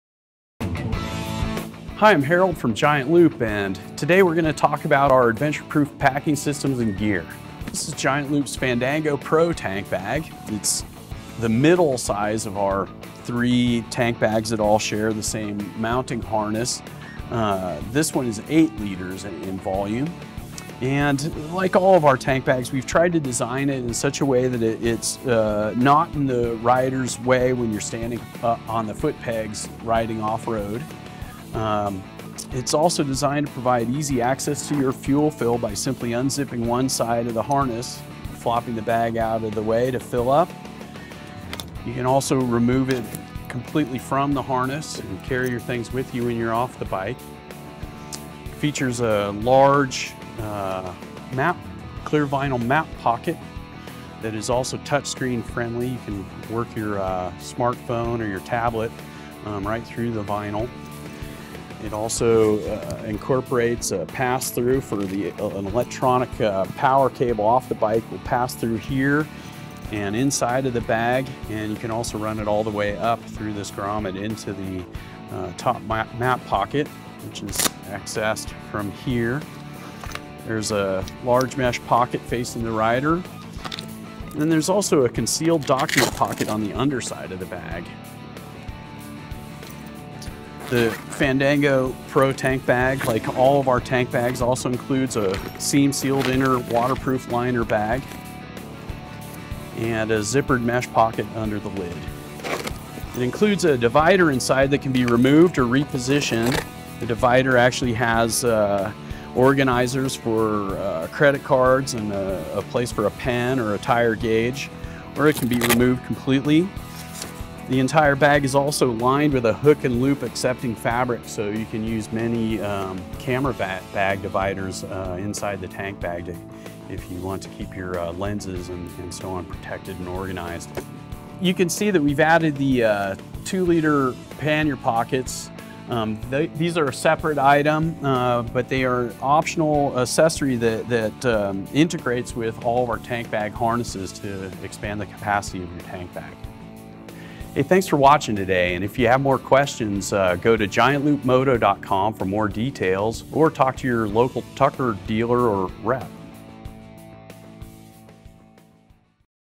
Giant Loop Fandango Pro Tank Bag
Fandango Tank Bag STANDARD (8 liters) is Giant Loop’s original adventure proof—tank bag. Easily access fuel fill by simply unzipping either side of tank bag from the harness, or completely remove bag leaving harness on bike, adjusted and ready to go. Concave tank bag front allows vent hoses to breathe. Available in BLACK only. Giant Loop direct web special: includes waterproof Dry Pod liner.